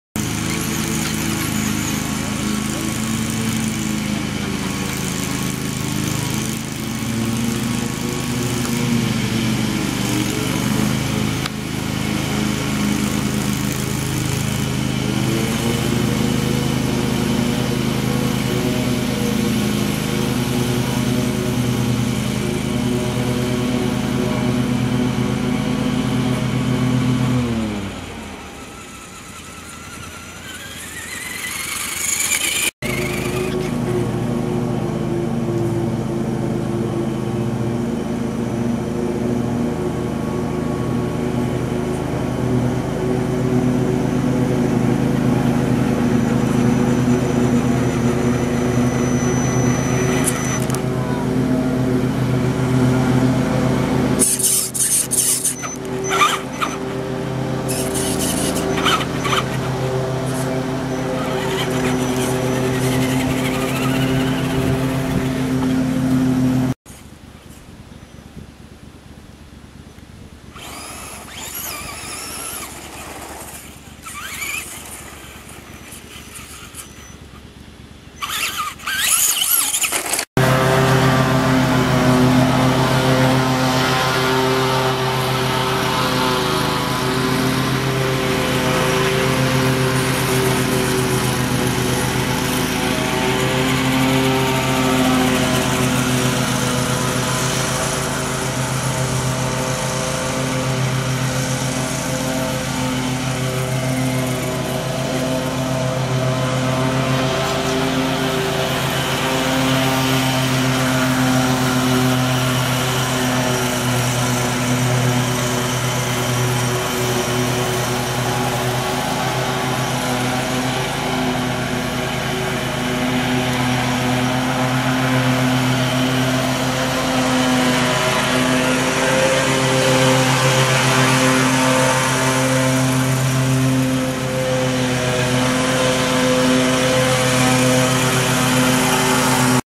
Fun N Sun Speedway
Bruce Nelson and his remote control car at the Fun N Sun Speedway. The Zoomers RC Club and track are open to all residents and guests of Fun N Sun Resort and other Encore Parks.